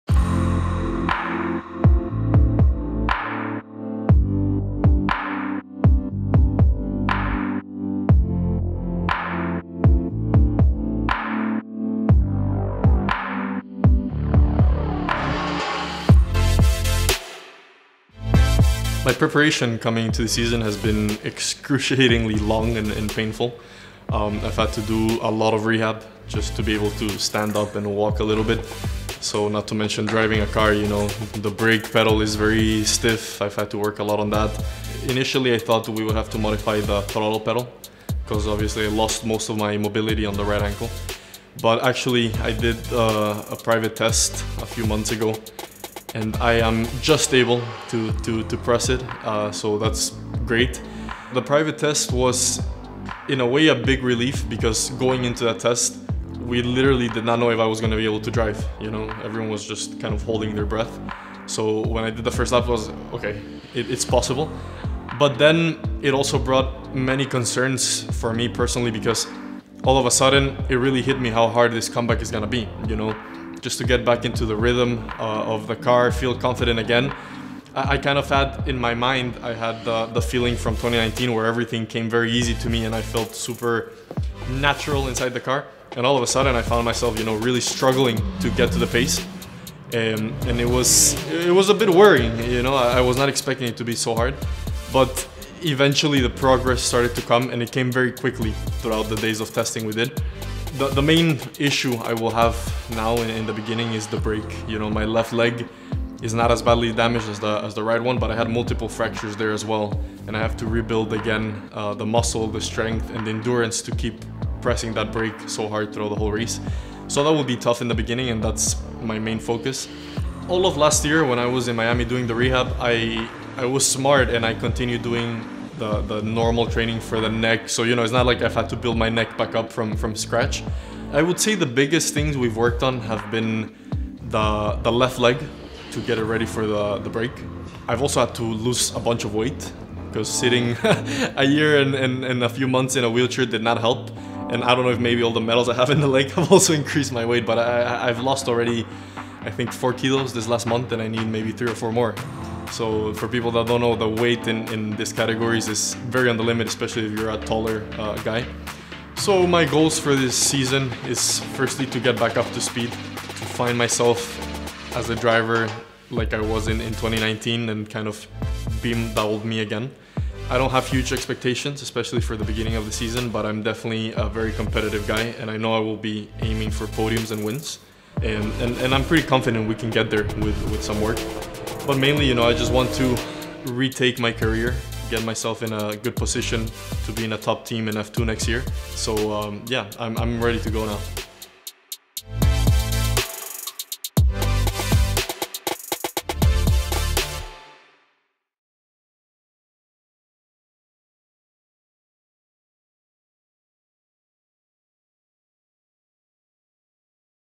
Juan Manuel Correa Prepares For Incredible Return To Racing
After a devastating crash at Spa in 2019, that claimed the life of fellow racer Anthoine Hubert, Juan Manuel Correa underwent numerous surgeries. Only 20 months later, he's preparing for a racing return in Formula 3...